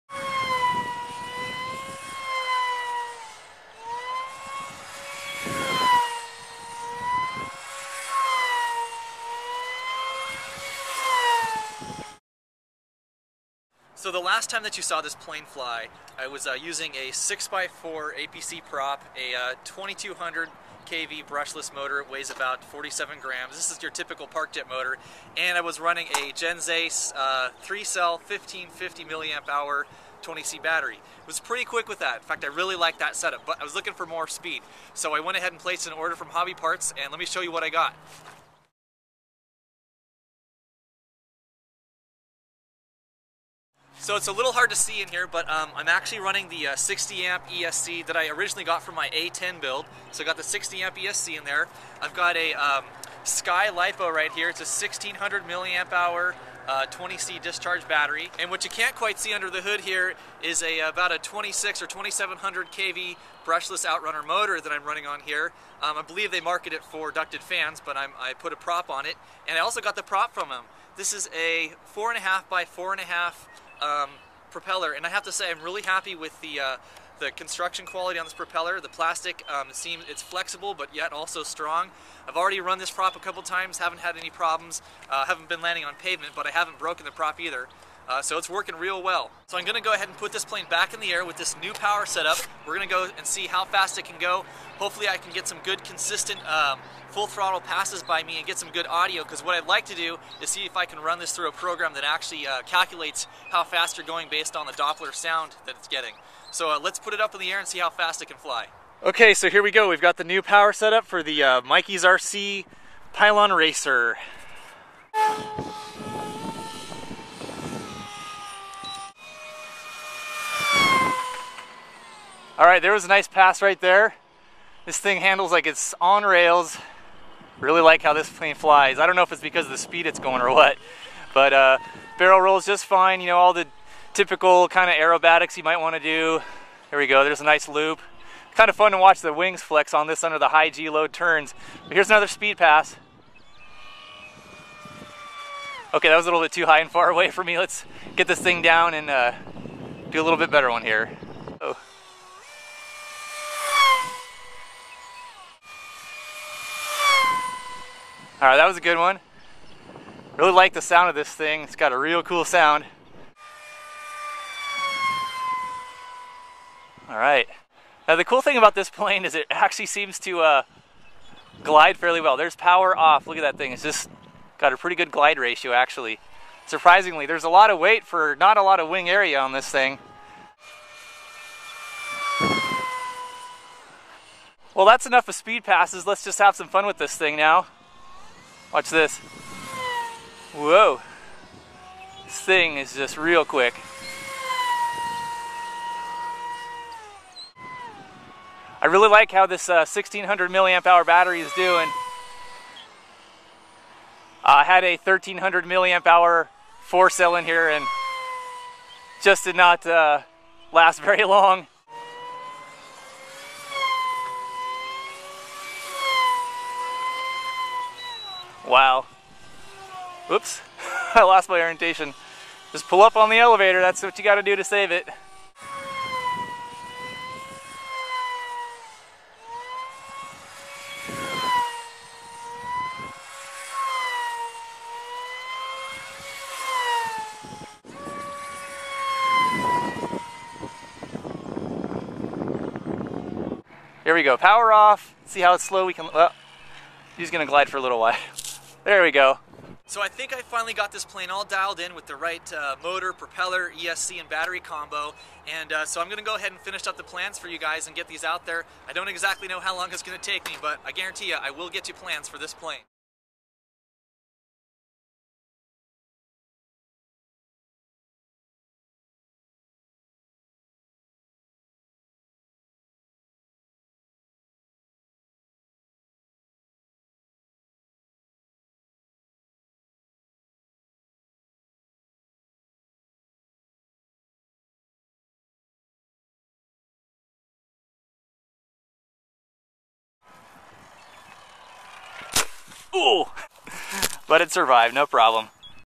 70MPH Scratch Built RC Plane $10 Airframe
This plane is made with foam board, hot glue, Popsicle sticks and 1/4 inch dowel!  All materials easy to find at your local craft store.

Link to the FREE Doplar speed measurement software tool.